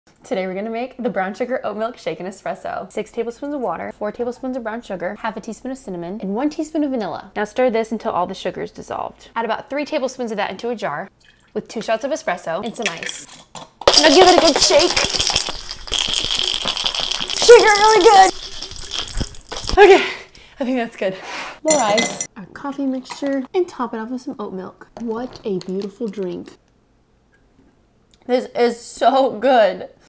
Brown Sugar Oat Milk Shaken Espresso AT HOME!!
Let me know if you want to see more Starbucks Drinks recipes you can make at home like this. Leave me your suggestions in the comments!

Wanna get my monthly ebook for FREE?!

The Caramel Frappuccino has been one of the most requested drinks on this channel so I"m so excited to teach you how to ska sit at home today. IT'S SO GOOD.

In this video Nicole Renard shows you how to make an easy Brown Sugar Oat Milk Shaken Espresso recipe from Starbucks. Want to see more Starbucks Drinks you can make at home?